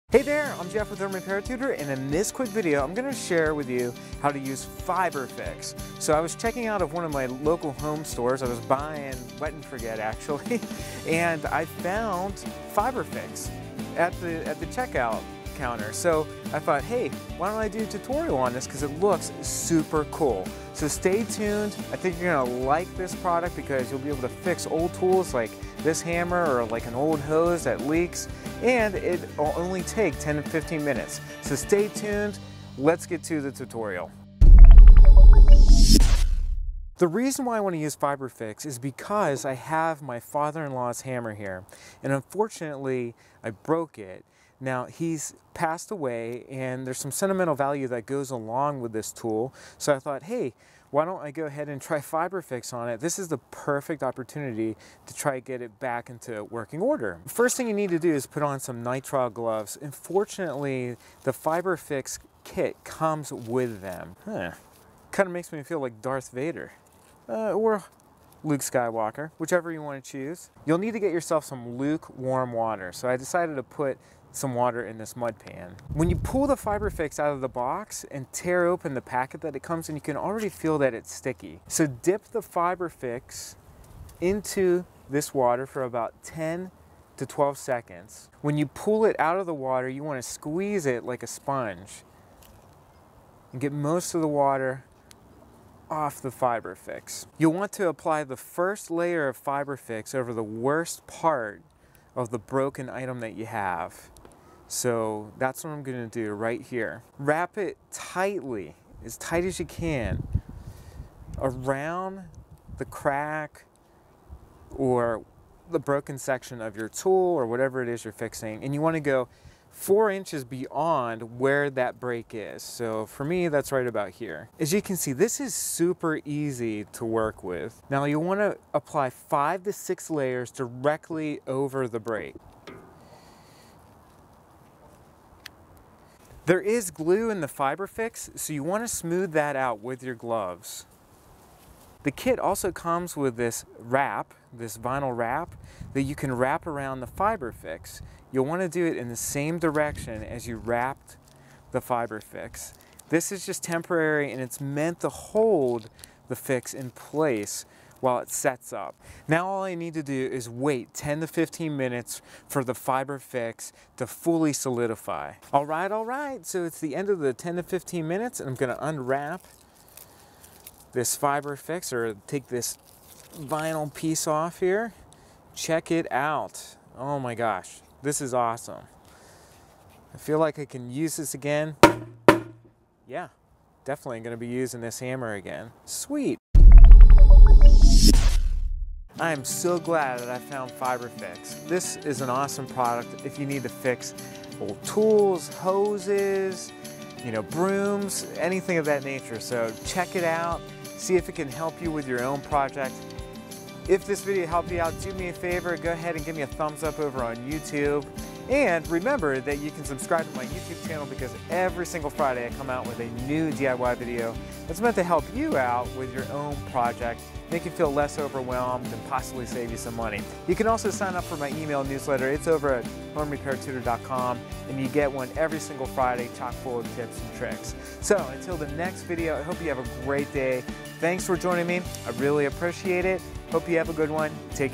How to Fix Anything with Fiber Fix
What's the easiest way to fix a broken shovel, chair, or garden hose? Maybe duct tape. But that only lasts for so long. I recently found an awesome solution for these problems called Fiber Fix and it's the coolest product I've ever used!!!

Yes that's bold but wait until you see how it repaired my broken hammer. After applying Fiber Fix and waiting 15 minutes my hammer was like new.

Here is a link to the product:
Fiber Fix

0:00 Getting started
1:37 Applying the product
2:21 The vinyl wrap
2:45 The finished product